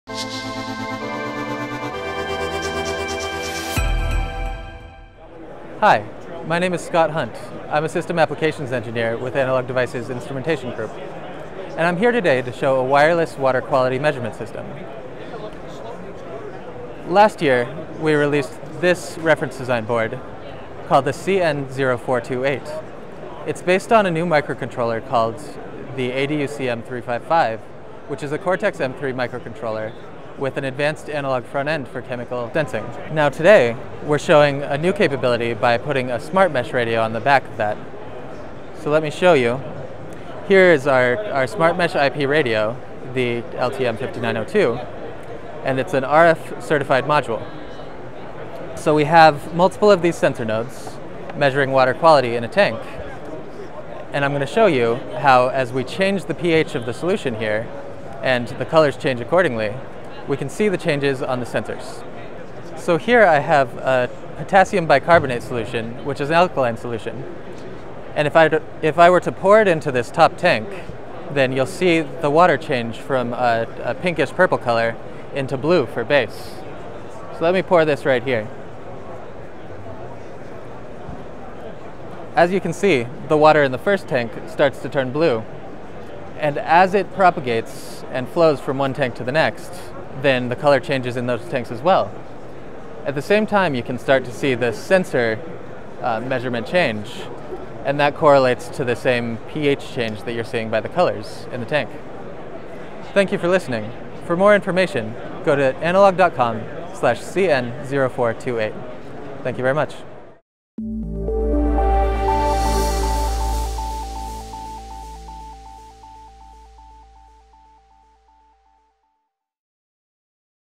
Analog Devices: Wireless Water Quality Measurement Featuring SmartMesh
See how the universal sensor interface capability of the ADuCM355 and the wire-like reliability of Analog Devices’ SmartMesh IP technology combine to form a robust and low-power wireless water quality monitoring system.